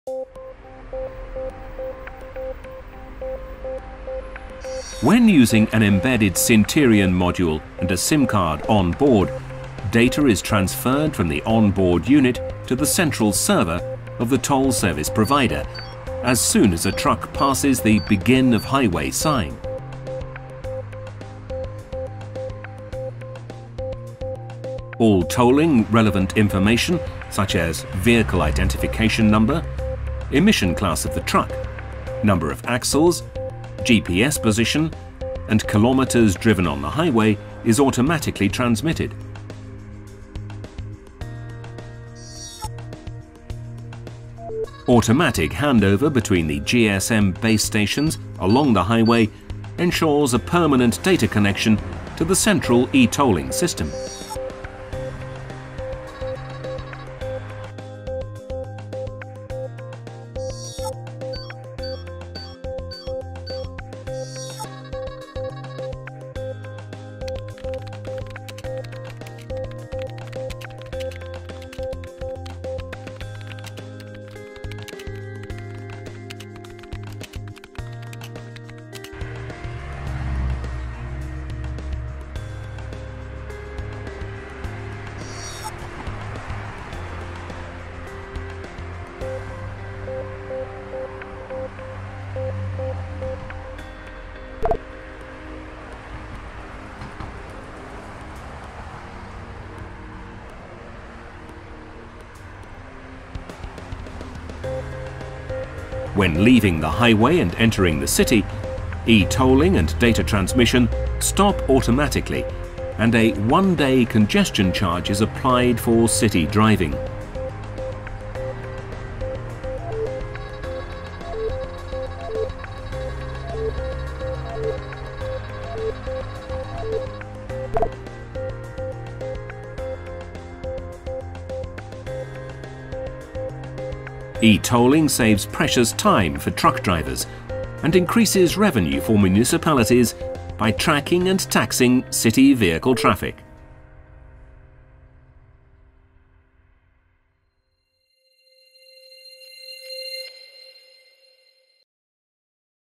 M2M Intelligent Transport Systems: eTolling
Watch Gemalto M2M´s Intelligent Transport Systems case study 'eTolling'.